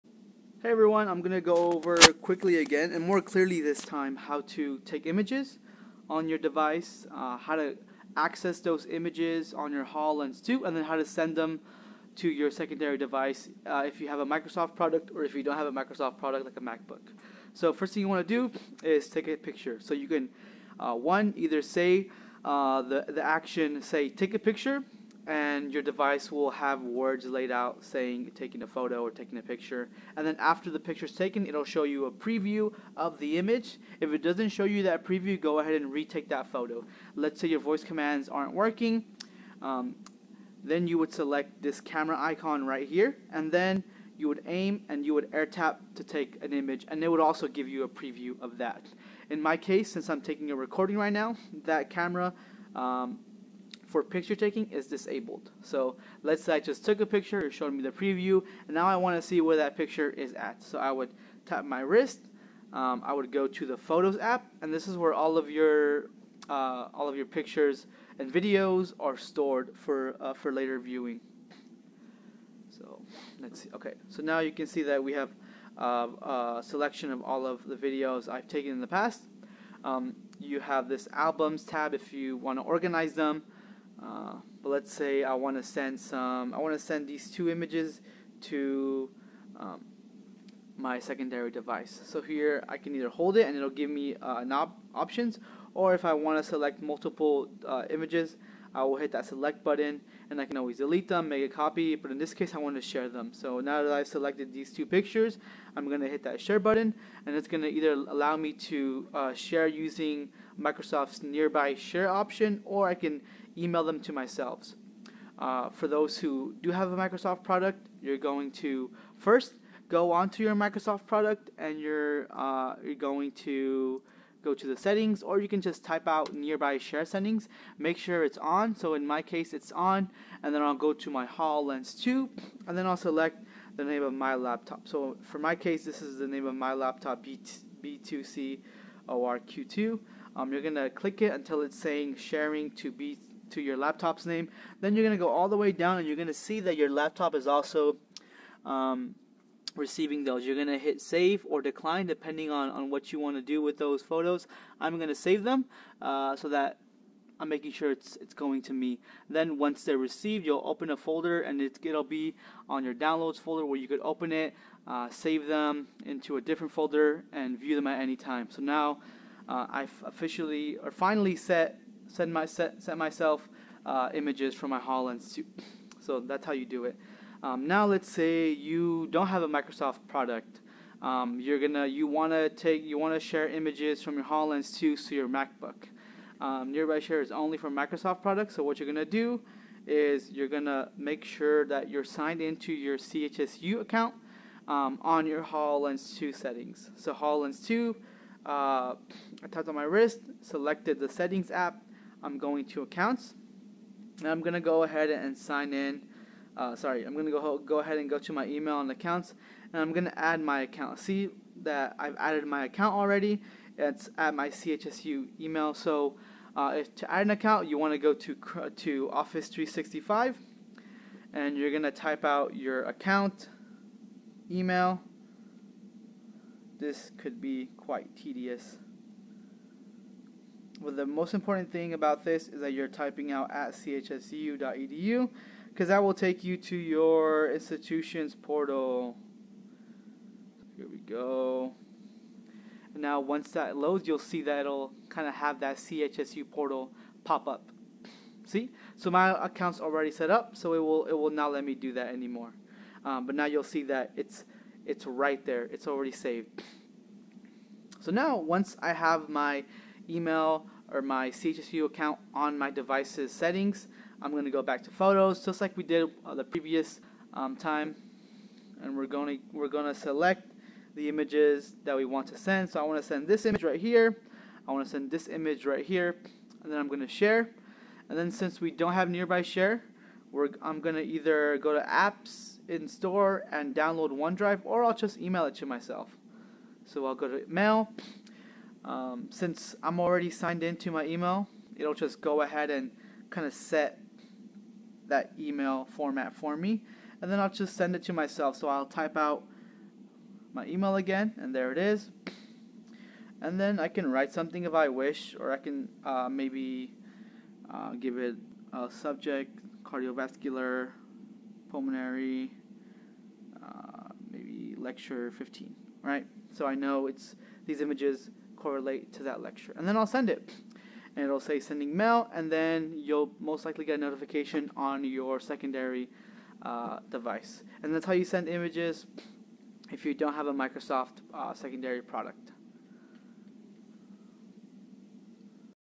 How can I send images from my HoloLens 2 to my laptop (Apple or Microsoft)
A quick video on how students using the HoloLens 2 can send images from their HoloLens 2 onto their secondary device (Apple or Microsoft based)

How to take a picture:
1. 0.00:19 to 0.00:56

How to send images from you HoloLens 2 to your Laptop:
1. Locating your pictures
• 0.00:56 to 0.01:05

2. Sharing Images to the 2nd Device (if 2nd Device is Microsoft Based)
• 0.01:05 to 0.02:58

3. Sharing Images to the 2nd Device (if 2nd Device is Apple Based)
• Adding your email account onto the device settings so that you may email yourself the images
• 0.02:58 to 0.05:35